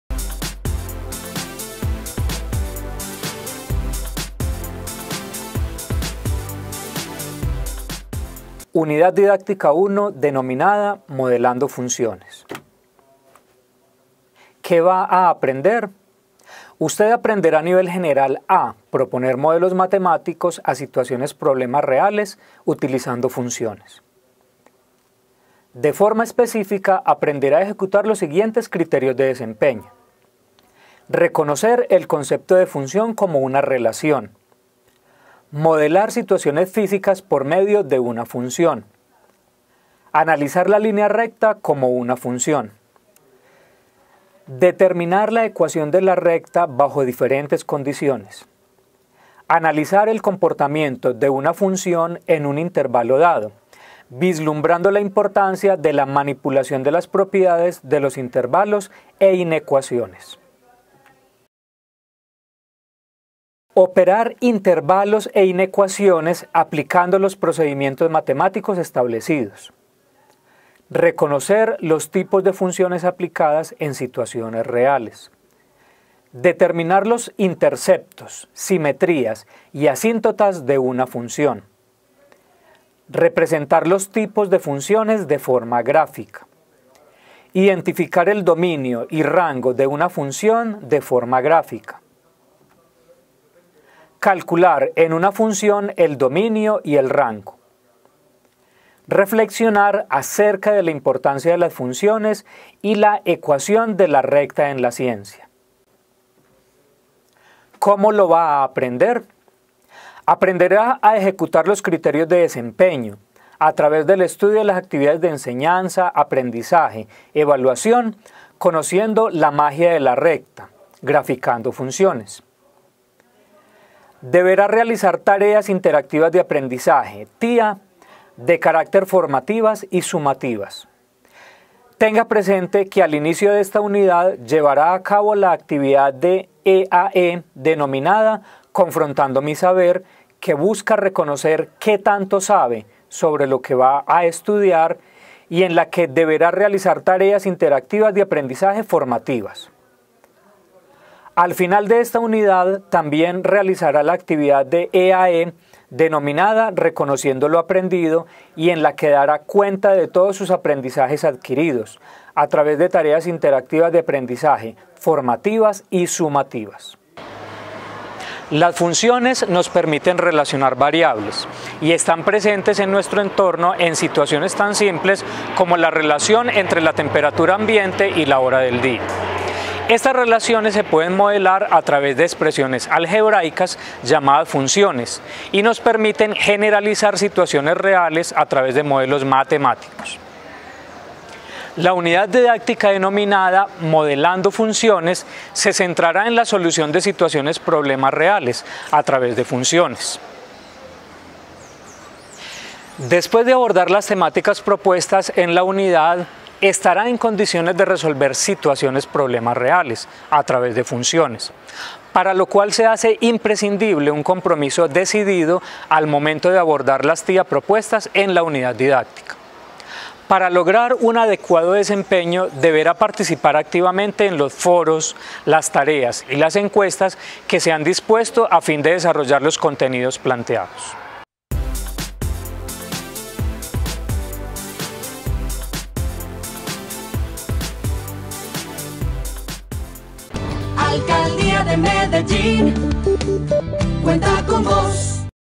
Cálculo Diferencial: UD1 - Modelando Funciones
Las funciones nos permiten relacionar variables y están presentes en nuestro entorno en situaciones tan simples como la relación entre la temperatura ambiente y la hora del día; estas relaciones se pueden modelar a través de expresiones algebraicas llamadas funciones y nos permiten generalizar situaciones reales a través a través de modelos matemáticos.

Coordinador

JEANETH GUTIERREZ RINCÓN
Pedagogos

Soporte Tecnológico

Diseño Gráfico

Producción Multimedial

Medellín Colombia
2017